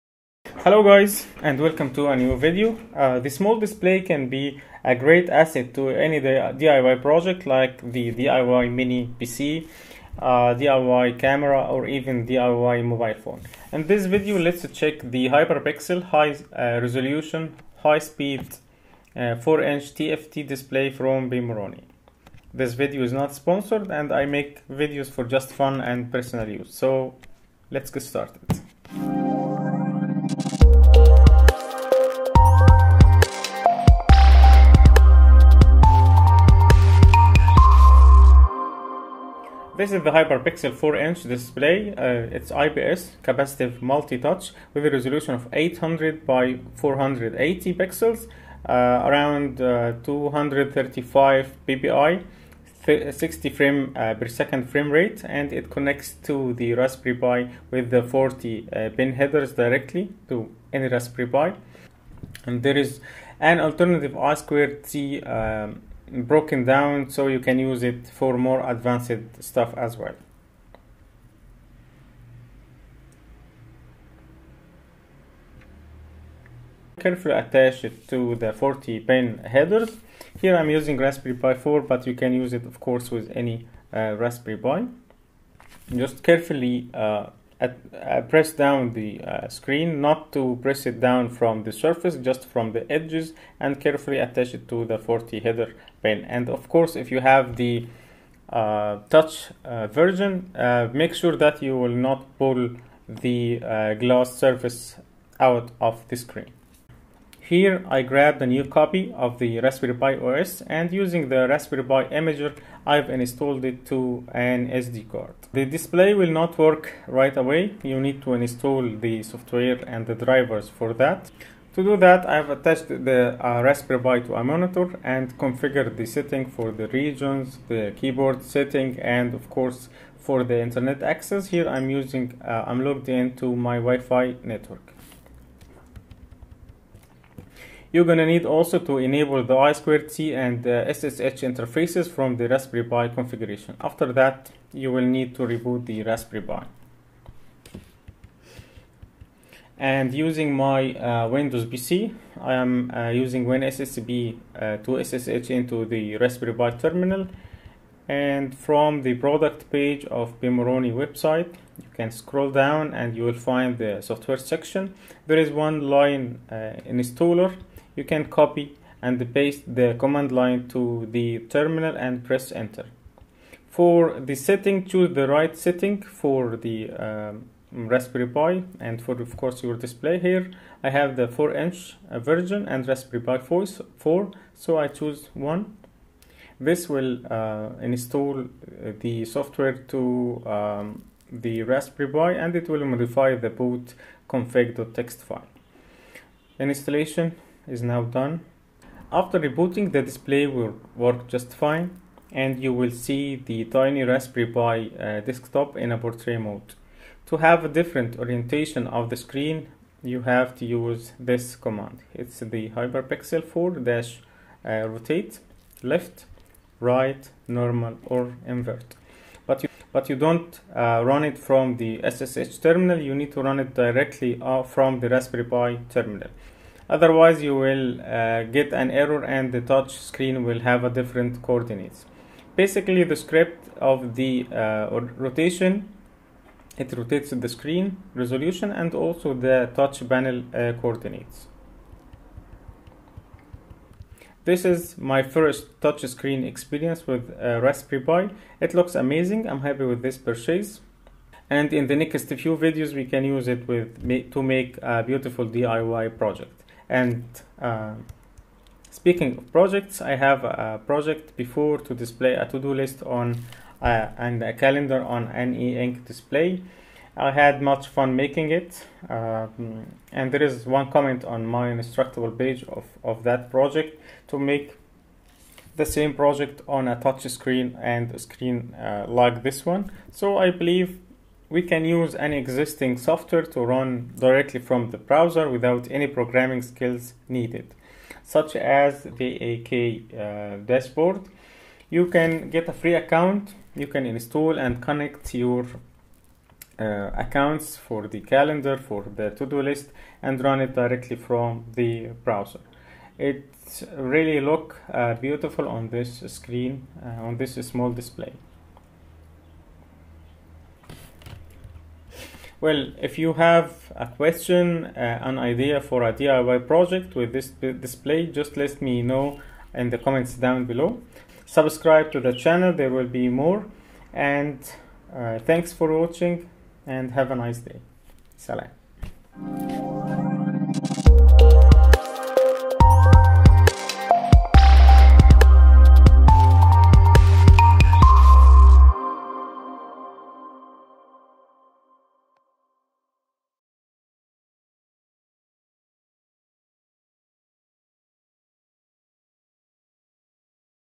HyperPixel 4.0 - Hi-Res Display for Raspberry Pi – Pimoroni
A high-resolution, high-speed 4.0" TFT display for your Raspberry Pi with optional capacitive multi-touch!